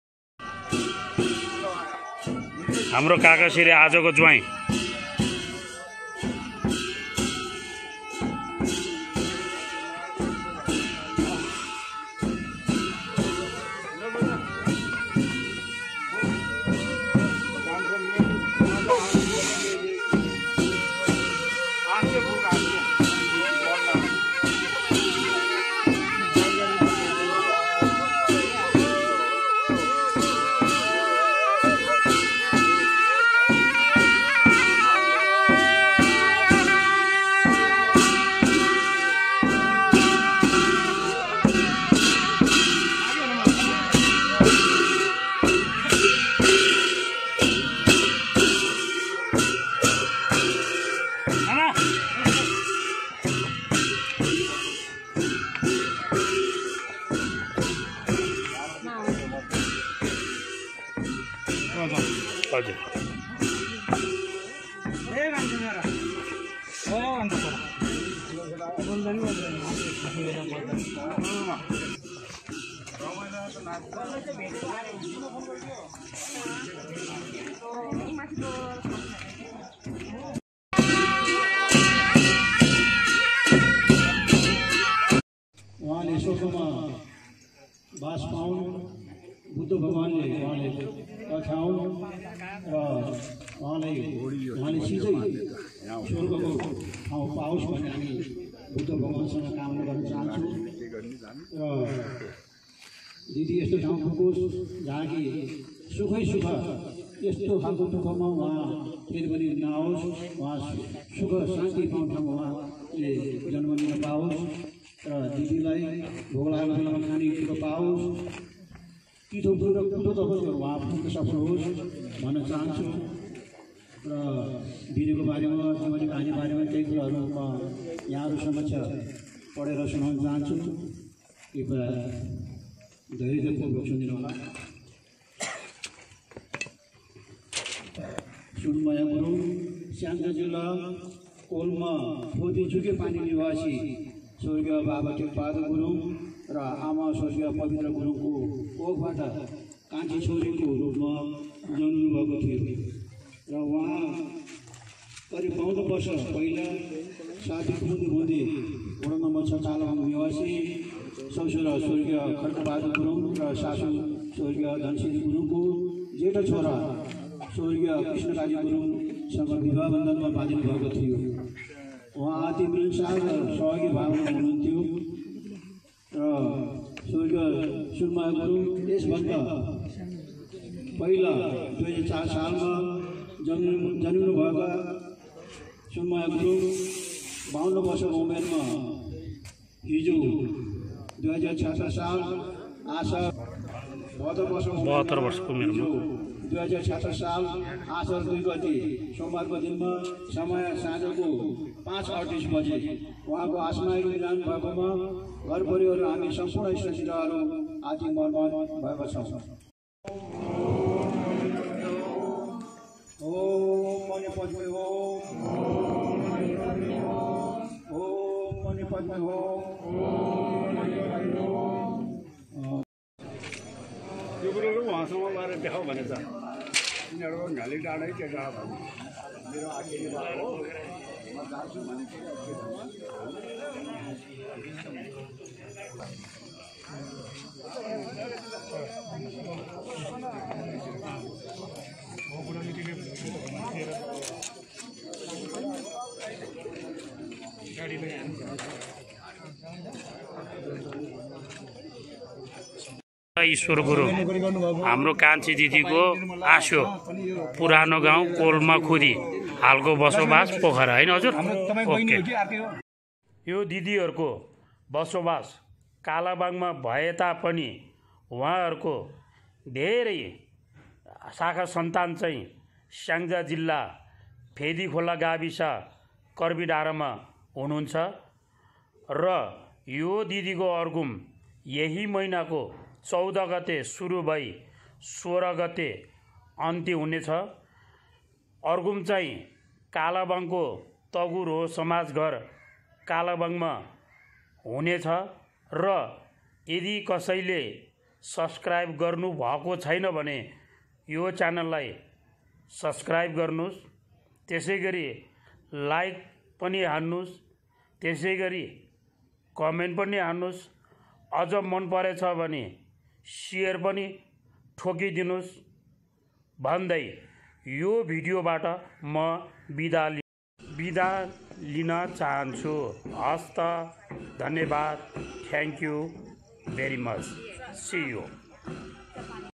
RIP Sunmaya Gurung || रहेनन् कालाबाङका सुनमाया गुरुङ पनि
गाउँले नाम हिराको ठागुको श्रीमती र उहाँको छोराको नाम बिक्रम गुरुङ हो । भाइखलक फेदिखोला कर्मिडाडा । माइत कोल्मा खुदी स्याङ्जा ।

Camara - Mimkaji Gurung
Editing - Sudhamsu Gurung